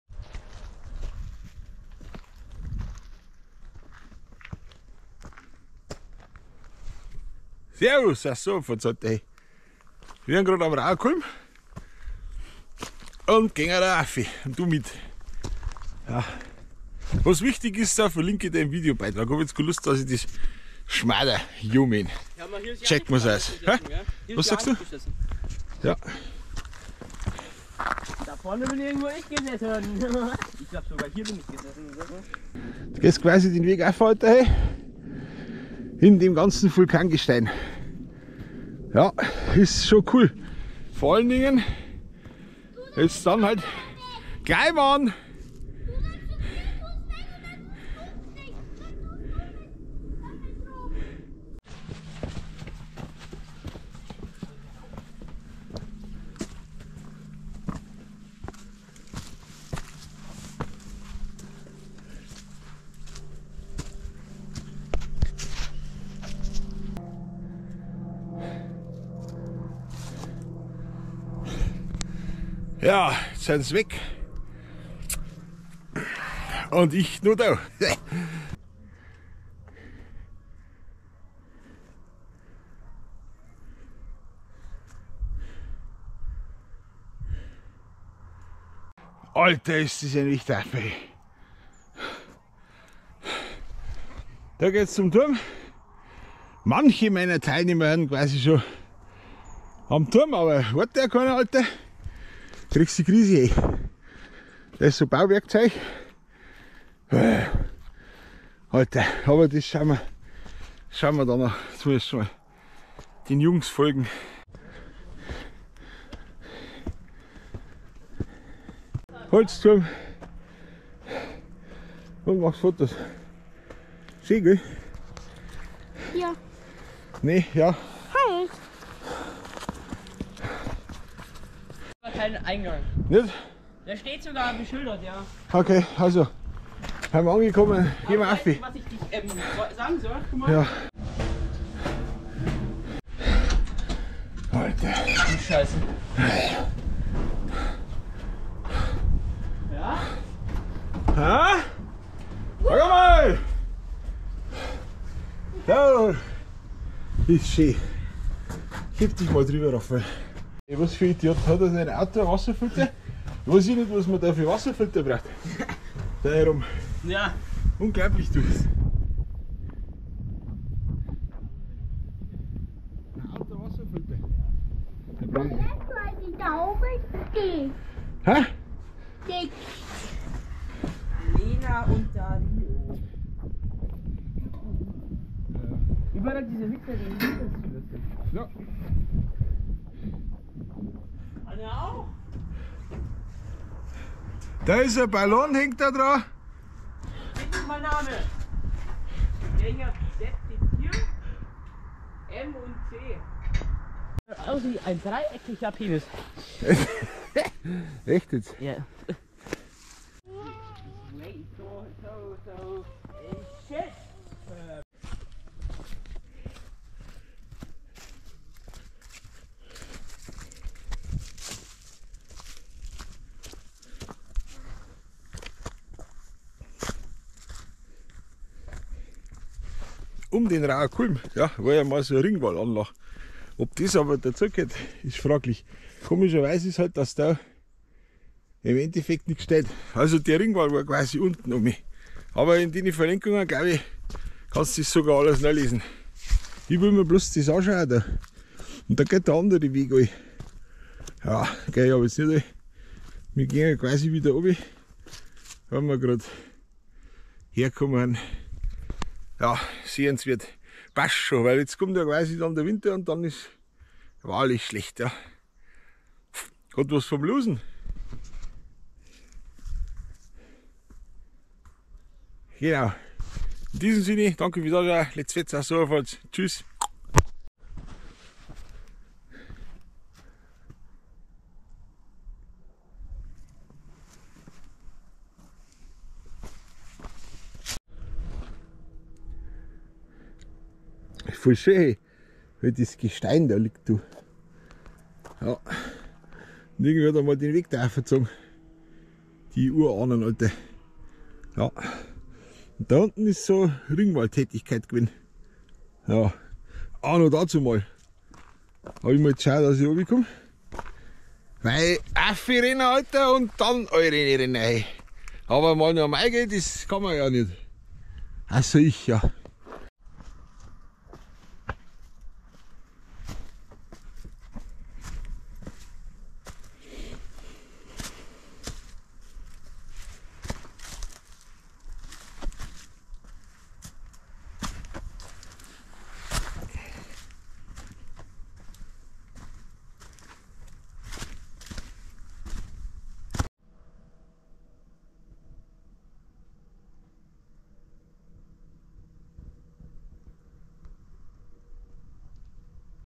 Raketenwanderung auf den Vulkan
vLog von der Wanderung auf den Rauher Kulm heute Nachmittag.

Schönes Wochenende und letz fetz aus der Oberpfalz.